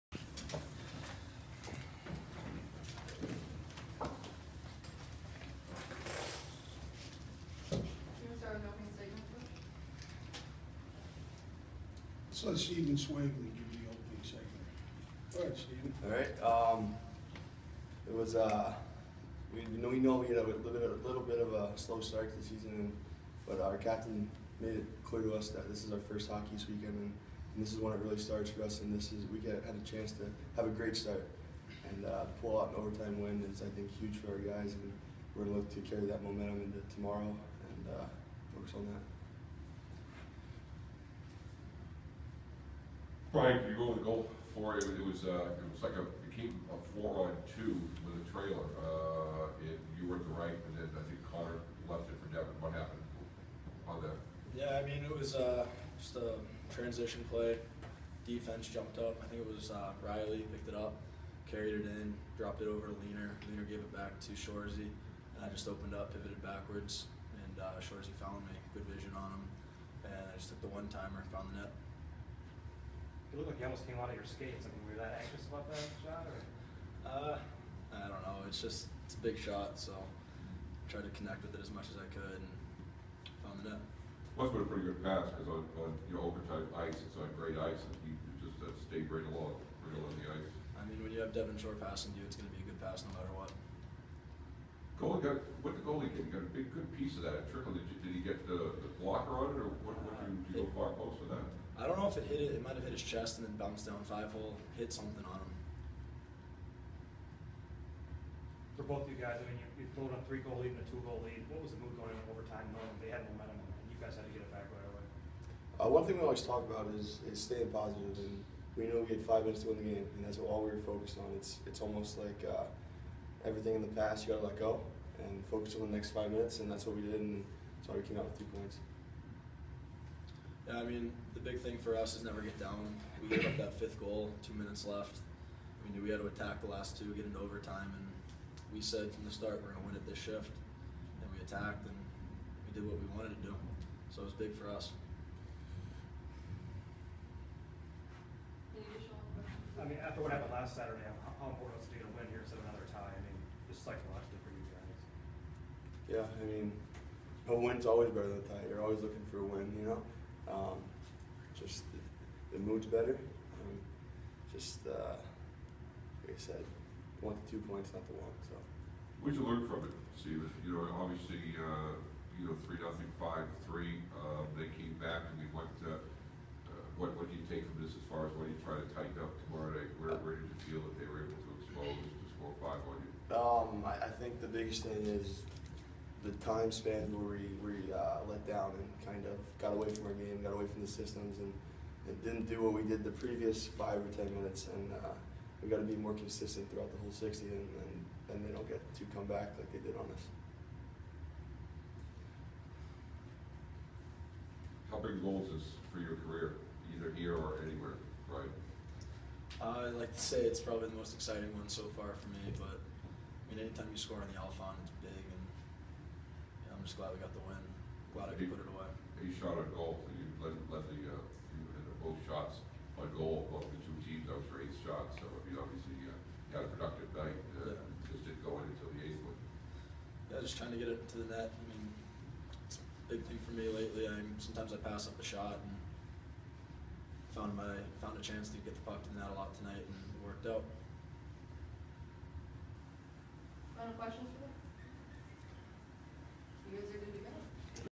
Steven Swavely & Brian Morgan, October 31, 2014
Steven Swavely and Brian Morgan speak to the media following Maine's 6-5 victory in OT over Umass